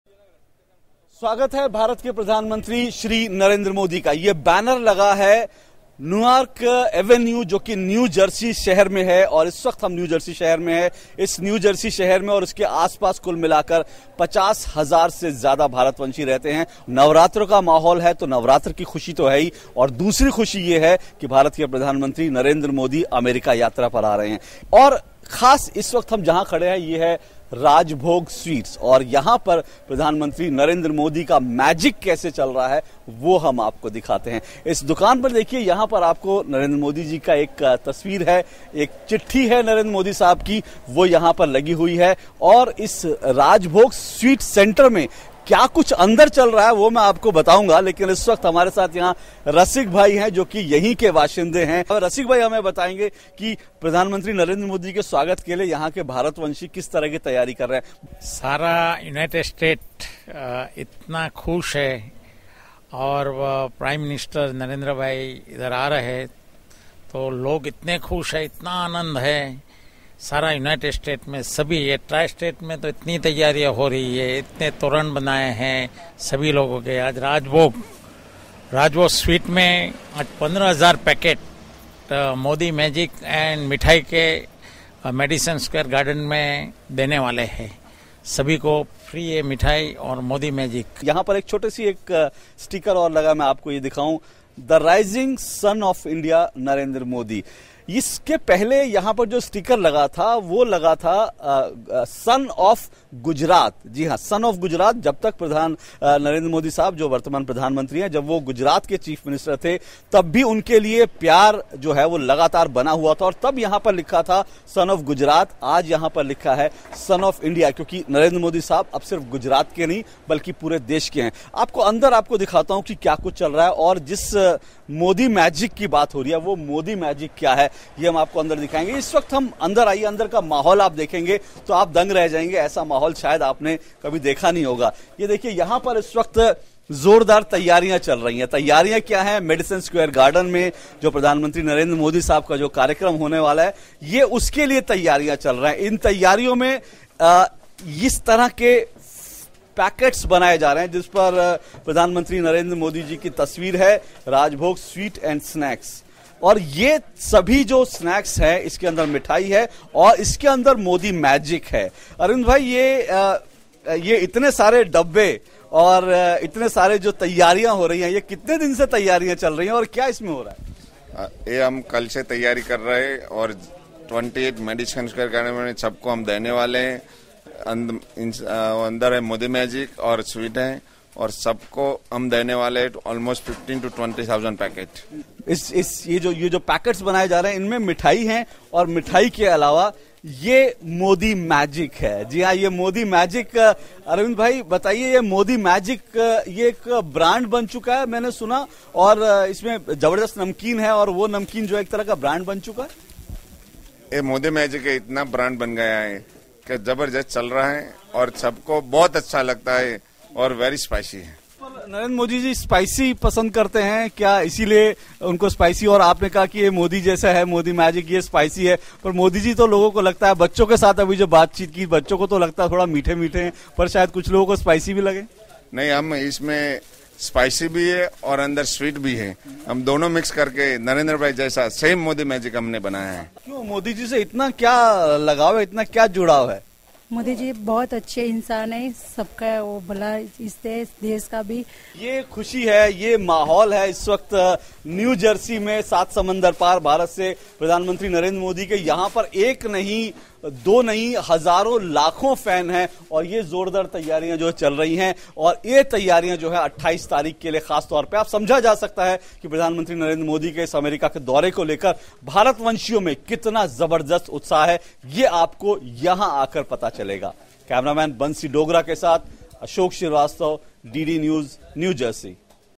New Jersey all geared up for PM Modi's visit
New Jersey all geared up for PM Narendra Modi's US visit. DD News  Correspondent Ashok Shrivastav takes you through all the preparations there.